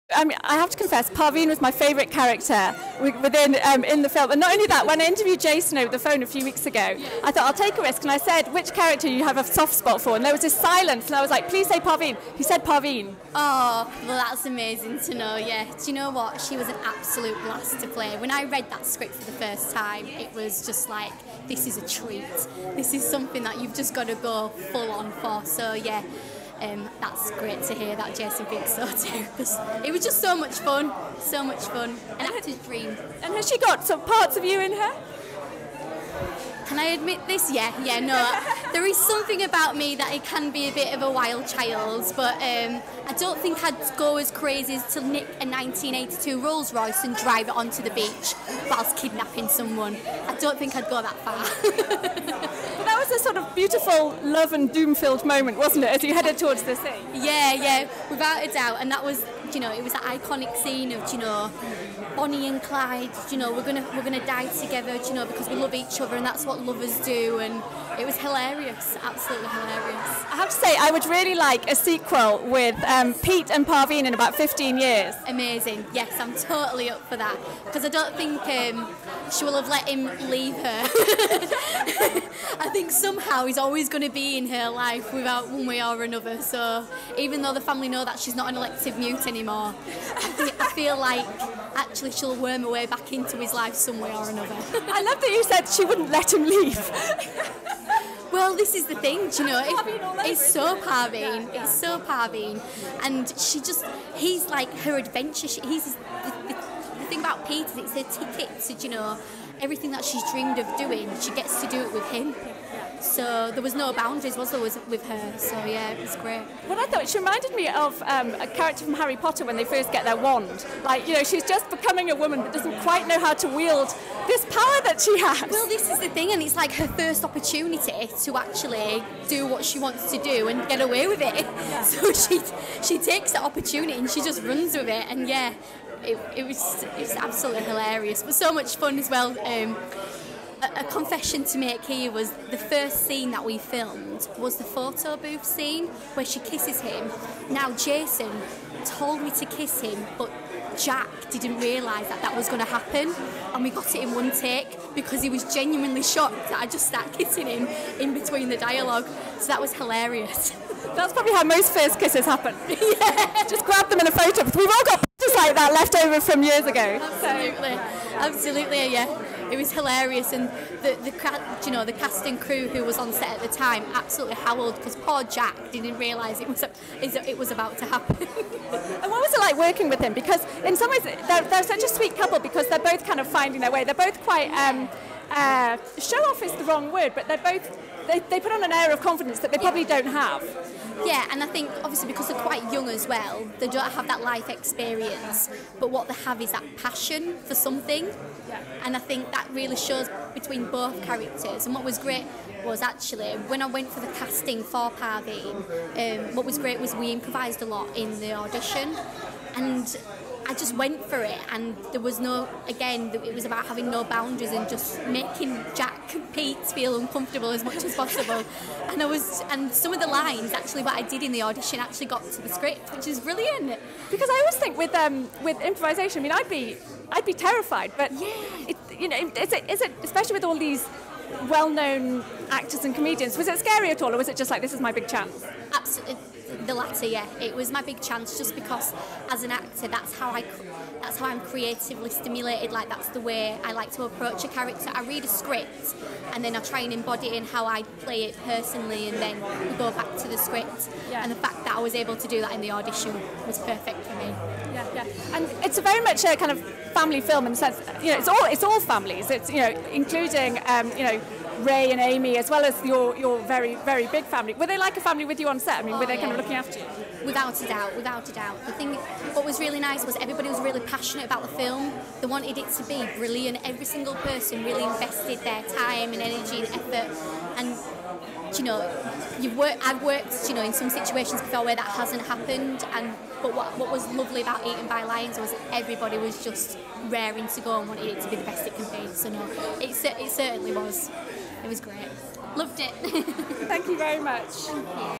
Natalie Davies (Parveen) interview - Eaten By Lions
Natalie Davies plays the teenage Parveen in British comedy Eaten By Lions. In a film full of terrific characters Parveen was my favourite (and Davies is wonderful in the role). She's a young woman who is utterly silent and seems a real introvert, but turns out to be very different (and enjoys the stirring of a relationship with Pete!) Eaten by Lions, about brothers Omar (Antonio Aakeel) and Pete (Jack Carroll) travelling to Blackpool in search of Omar's long lost dad, is a hoot and opens in the UK this Friday (9 March).